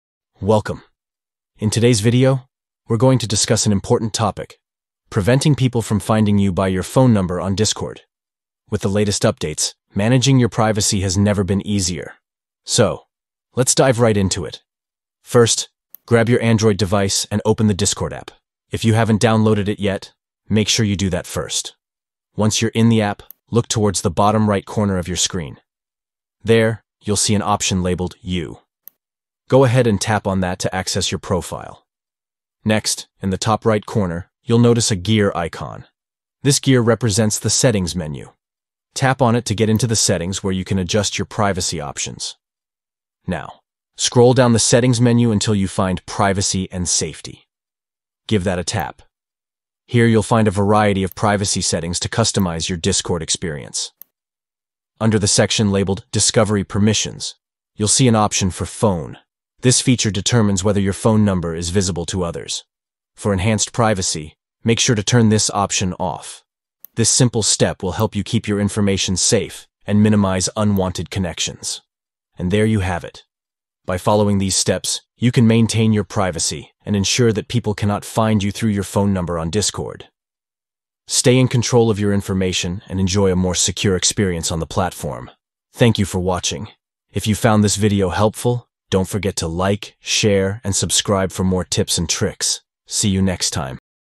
How to Prevent People From Finding You By Your Phone Number on Discord (Updates)
Learn how to prevent people from finding you by your phone number on Discord with this straightforward guide. Keeping your privacy intact is essential, especially on platforms like Discord. Follow these simple steps to secure your account and enjoy a safer online experience.
Steps:

1. Open the Discord app on your Android device.
2. Tap "You" at the bottom right.
3. Tap the gear icon at the top right to access settings.
4. Scroll down and then tap "Privacy & Safety".
5. Under "Discovery Permissions", turn off "Phone".

Now you can chat and connect on Discord without worrying about your phone number being accessed by others. Stay safe and enjoy your Discord experience.